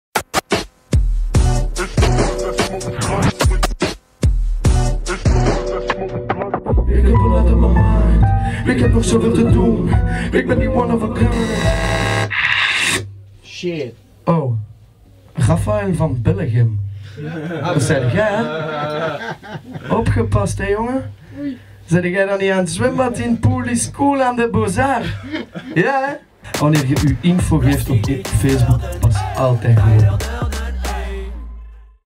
Gratis concert van Zwangere Guy in ruil voor je facebookgegevens !
Bye bye persoonlijke gegevens!